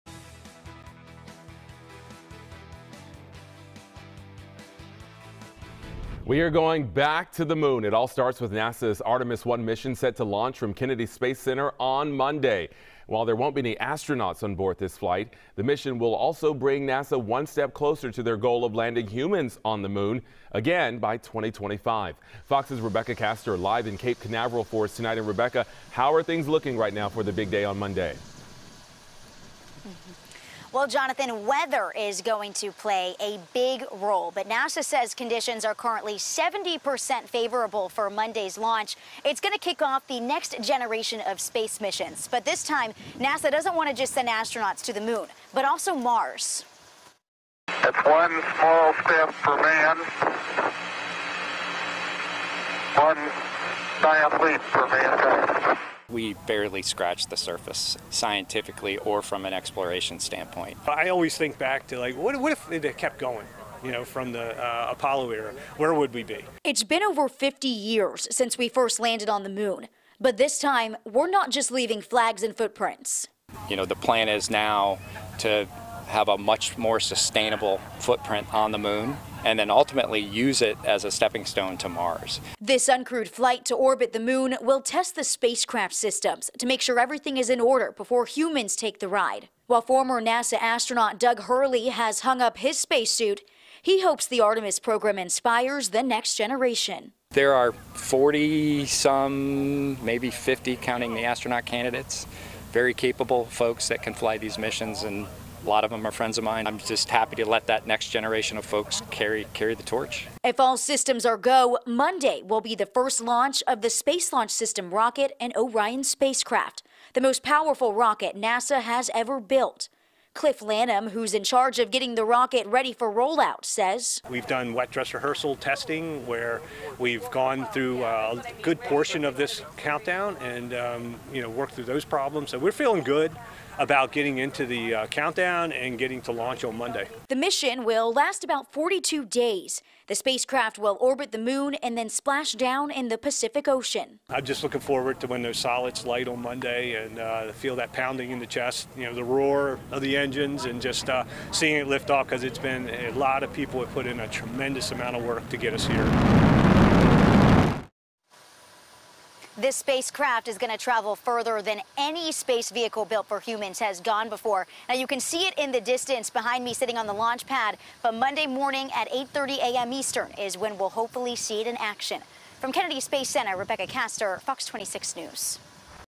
Artemis I: NASA's moon rocket is on the launch pad
We’re going back to the moon. It all starts with NASA's Artemis I Mission, which is set to launch from Kennedy Space center on Monday.